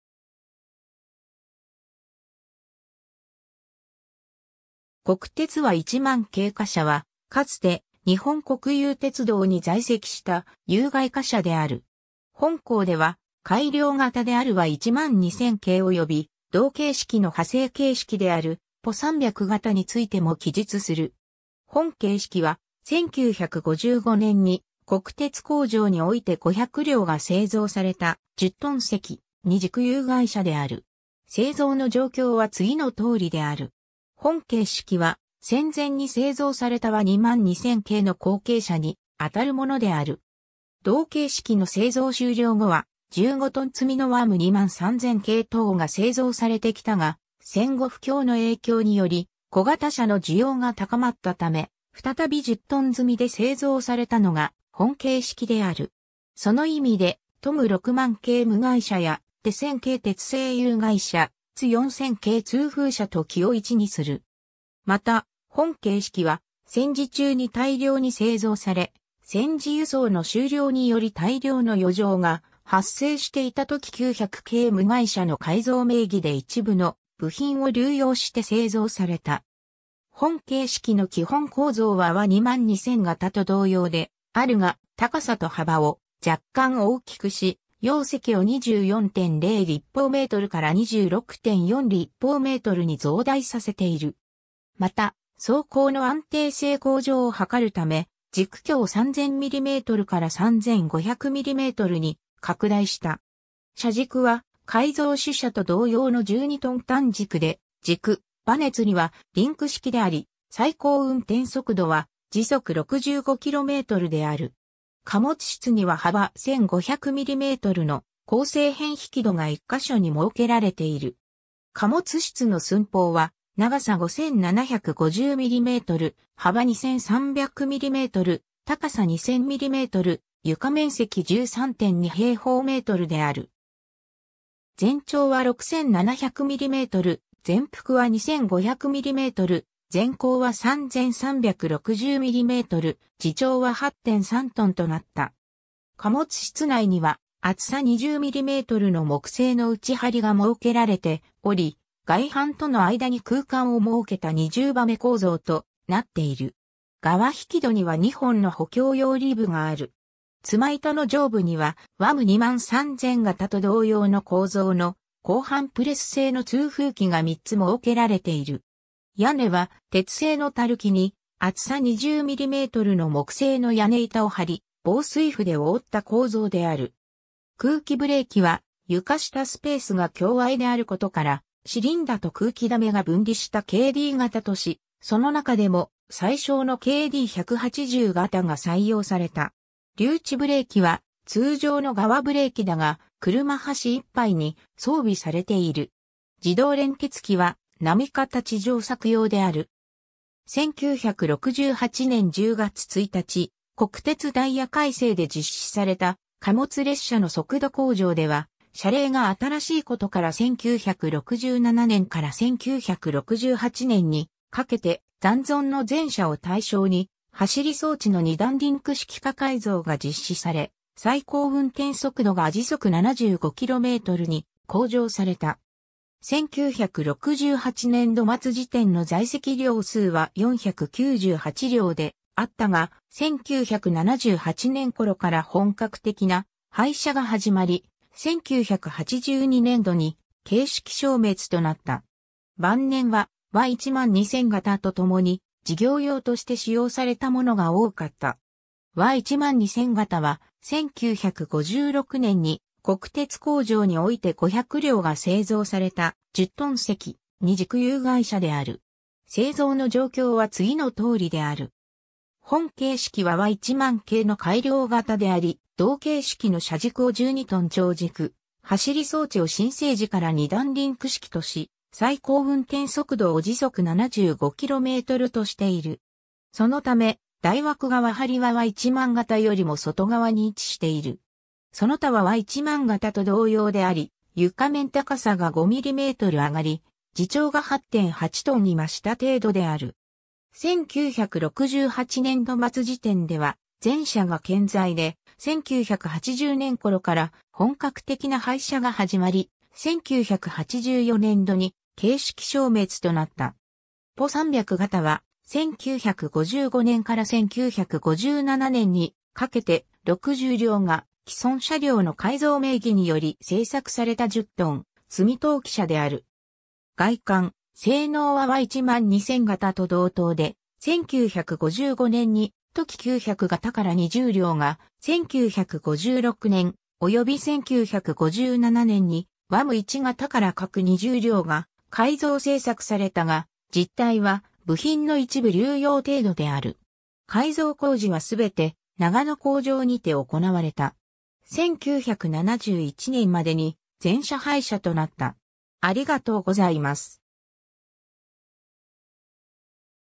国鉄ワ10000形貨車
国鉄ワ10000形貨車（こくてつワ10000がたかしゃ）は、かつて日本国有鉄道（国鉄）に在籍した有蓋貨車である。
本項では、改良型であるワ12000形および同形式の派生形式であるポ300形についても記述する。
 本形式は、1955年（昭和30年）に、国鉄工場において500両（ワ10000 - ワ10499）が製造された、10トン積二軸有蓋車である。
製造の状況は次のとおりである。
 本形式は、戦前に製造されたワ22000形の後継車にあたるものである。
同形式の製造終了後は、15トン積みのワム23000形等が製造されてきたが、戦後不況の影響により、小型車の需要が高まったため、再び10トン積みで製造されたのが本形式である。
その意味で、トム60000形無蓋車やテ1000形鉄製有蓋車、ツ4000形通風車と軌を一にする。
また、本形式は、戦時中に大量に製造され、戦時輸送の終了により大量の余剰が発生していたトキ900形無蓋車の改造名義で一部の部品を流用して製造された。
 本形式の基本構造はワ22000形と同様であるが、高さと幅を若干大きくし、容積を24.0m3から26.4m3に増大させている。
また、走行の安定性向上を図るため、軸距を3,000mmから3,500mmに拡大した。
車軸は改造種車と同様の12トン短軸で、軸ばね吊り（走り装置）は（一段）リンク式であり、最高運転速度は65km/hである。
 貨物室には幅1,500mmの鋼製片引戸が1か所（片側）に設けられている。
貨物室の寸法は、長さ5,750mm、幅2,300mm、高さ2,000mm、床面積13.2m2である。
全長は6,700mm、全幅は2,500mm、全高は3,360mm、自重は8.3tとなった。
貨物室内には厚さ20mmの木製の内張りが設けられており、外板との間に空間を設けた二重羽目構造となっている。
側引き戸には2本の補強用リブがある。
妻板の上部には、ワム23000形と同様の構造の鋼板プレス製の通風器が3つ設けられている。
屋根は、鉄製の垂木に厚さ20mmの木製の屋根板を張り、防水布で覆った構造である。
 空気ブレーキは、床下スペースが狭隘であることから、シリンダと空気溜めが分離したKD形とし、その中でも最小のKD180形が採用された。
留置ブレーキは通常の側ブレーキだが、車端一杯に装備されている。
自動連結器は並形上作用である。
 1968年（昭和43年）10月1日国鉄ダイヤ改正で実施された貨物列車の速度向上では、車齢が新しいことから1967年（昭和42年）から1968年（昭和43年）にかけて残存の全車を対象に、走り装置の二段リンク式化改造が実施され、最高運転速度が75km/hに向上された。
1968年（昭和43年）度末時点の在籍両数は498両であったが、1978年（昭和53年）頃から本格的な廃車が始まり、1982年（昭和57年）度に形式消滅となった。
晩年は、ワ12000形とともに事業用（配給車や救援車）として使用されたものが多かった。
 ワ12000形は、1956年（昭和31年）に国鉄工場において500両（ワ12000 - ワ12499）が製造された、10トン積二軸有蓋車である。
 本形式はワ10000形の改良型であり、同形式の車軸を12トン長軸、走り装置を新製時から二段リンク式とし、最高運転速度を75km/hとしている。
そのため、台枠側梁はワ10000形よりも外側に位置している。
その他はワ10000形と同様であり、床面高さ（全高）が5mm上がり、自重が8.8トンに増した程度である。
 1968年度末時点では全車が健在で、1980年（昭和55年）頃から本格的な廃車が始まり、1984年（昭和59年）度に形式消滅となった。
 ポ300形は、1955年（昭和30年）から1957年（昭和32年）にかけて、60両（ポ300 - ポ359）が既存車両の改造名義により製作された10トン積み陶器車である。
外観・性能はワ12000形と同等で、1955年にトキ900形から20両（ポ300 - ポ319）が、1956年（昭和31年）および1957年（昭和32年）にワム1形から各20両（計40両。ポ320 - ポ359）が改造製作されたが、実態は部品の一部流用程度である。
改造工事はすべて長野工場にて行われた。
1971年（昭和46年）までに全車廃車となった。